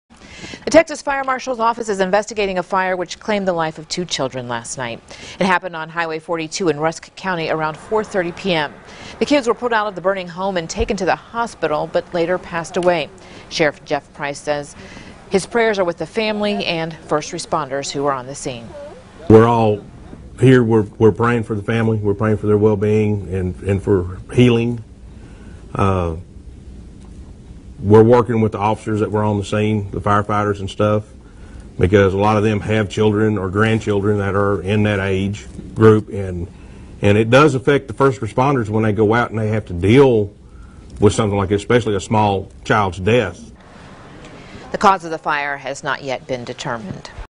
House fire claims the lives of 2 children in East Texas
Two children are dead after fire rips through their East Texas home.

The Texas Fire Marshal's Office has been called to investigate the blaze in Rusk County.

According to the Overton Fire Department, the fire broke out Wednesday afternoon at a home on Hwy 42, near the intersection of Hwy 64 E.

Sgt. David Roberts of Rusk County said the fire occurred around 4:30 p.m. and confirmed that three deputies pulled two children from the house, and put them in an ambulance.

The children were taken to a Henderson hospital, where they later died.